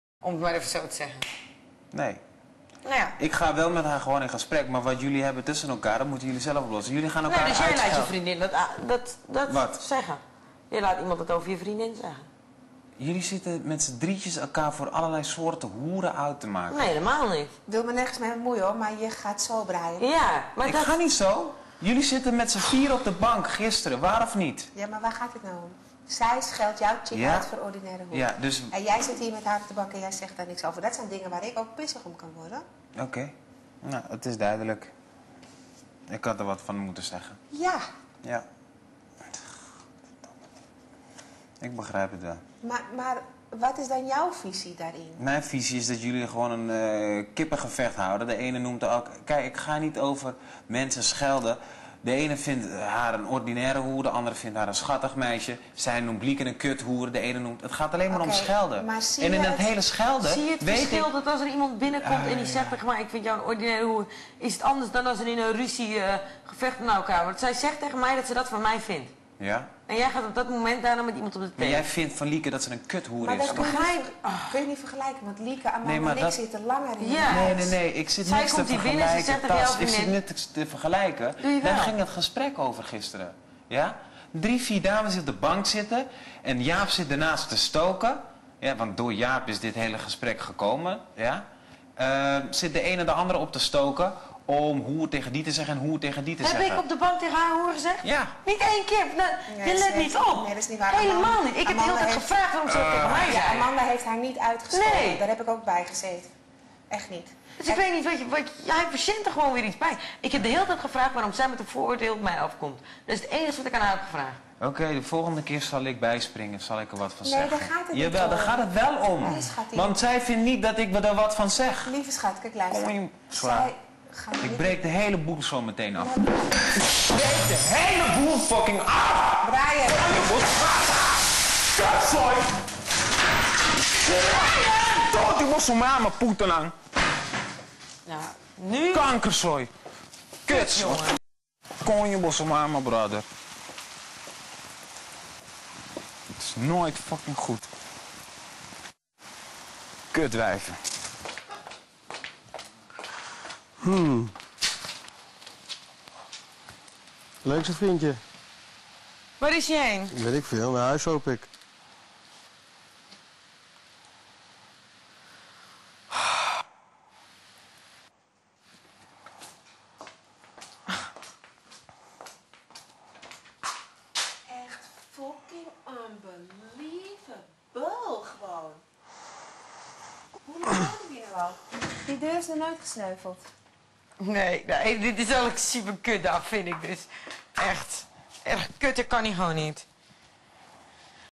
Brian Agressief en slaat glazen deur kapot
Brian is agressief na een discussie met Amanda en Natasia en Slaat uit woede een deur kapot